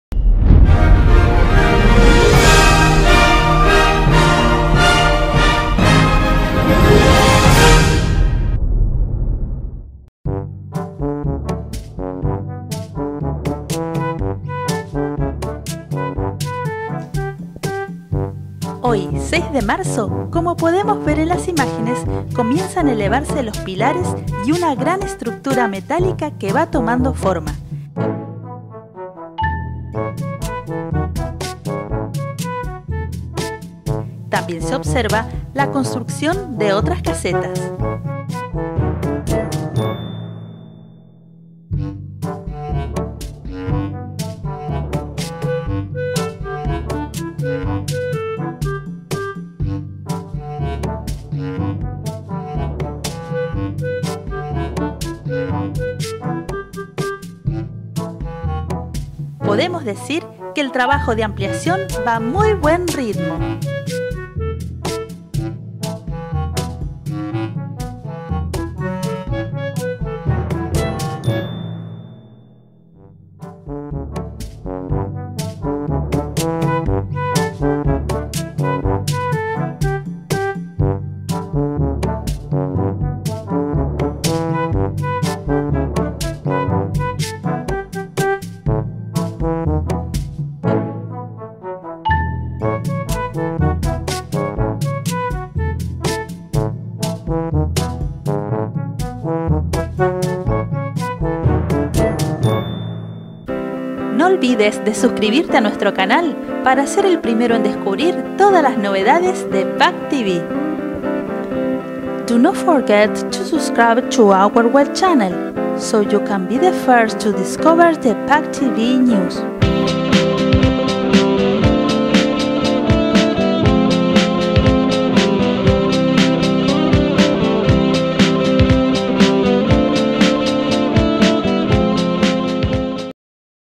Diario de Construcción PortAventura Aquatic 2013
Nuevo Informe de la ampliación del Costa Caribe Aquatic Parck de PortAventura